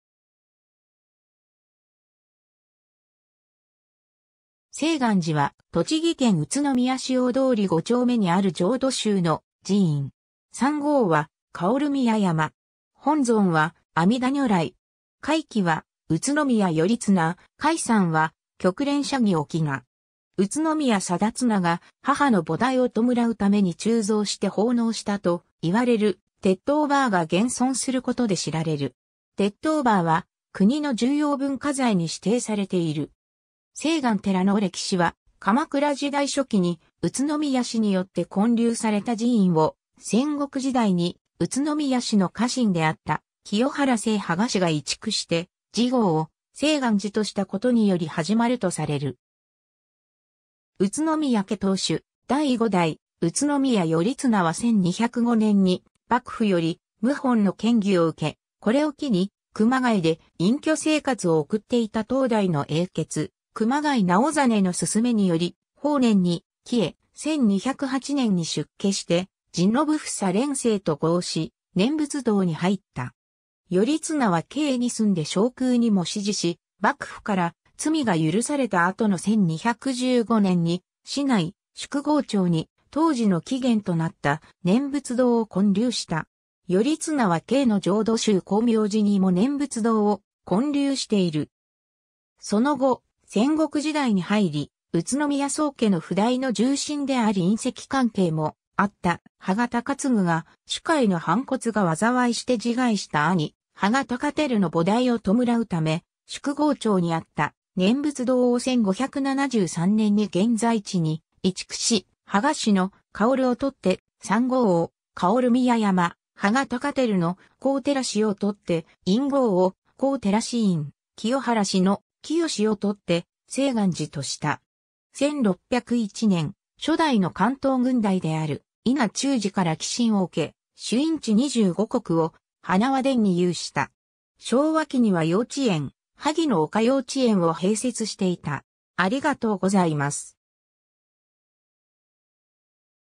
清巌寺
清巌寺（せいがんじ）は、栃木県宇都宮市大通り五丁目にある浄土宗の寺院。
山号は芳宮山。
本尊は阿弥陀如来（あみだにょらい）。
開基（創立者）は宇都宮頼綱、開山は旭蓮社儀翁。
宇都宮貞綱が母の菩提を弔うために鋳造して奉納したと謂われる鉄塔婆が現存することで知られる。
鉄塔婆は国の重要文化財に指定されている。
 清巌寺の歴史は、鎌倉時代初期に宇都宮氏によって建立された寺院を、戦国時代に宇都宮氏の家臣であった清原姓芳賀氏が移築して寺号を清巌寺としたことにより始まるとされる。
 宇都宮家当主・第5代宇都宮頼綱は1205年（元久2年）に幕府より謀反の嫌疑を受け、これを機に熊谷で隠居生活を送っていた当代の英傑・熊谷直実（熊谷蓮生入道）の勧めにより法然に帰依、1208年（承元2年）に出家して実信房蓮生と号し念仏道に入った。
頼綱は京に住んで証空にも師事し、幕府から罪が許された後の1215年（建保3年）に市内・宿郷町に当寺の起源となった念仏堂を建立した。
頼綱は京の浄土宗光明寺にも念仏堂を建立している（現在の京都市右京区常盤の西方寺といわれる）。
 その後、戦国時代に入り、宇都宮宗家の譜代の重臣であり姻戚関係もあった芳賀高継が、主家への反骨が災いして自害した兄・芳賀高照の菩提を弔うため、宿郷町にあった念仏堂を1573年（天正元年）に現在地に移築し、「芳賀氏」の「芳」を採って山号を「芳宮山」、「芳賀高照」の「高照」を採って院号を「高照院」、「清原氏」の「清」を採って「清巌寺」とした。
 1601年（慶長3年）、初代の関東郡代である伊奈忠次から寄進を受け、朱印地25石を塙田に有した。
 昭和期には幼稚園「萩の丘幼稚園」を併設していた。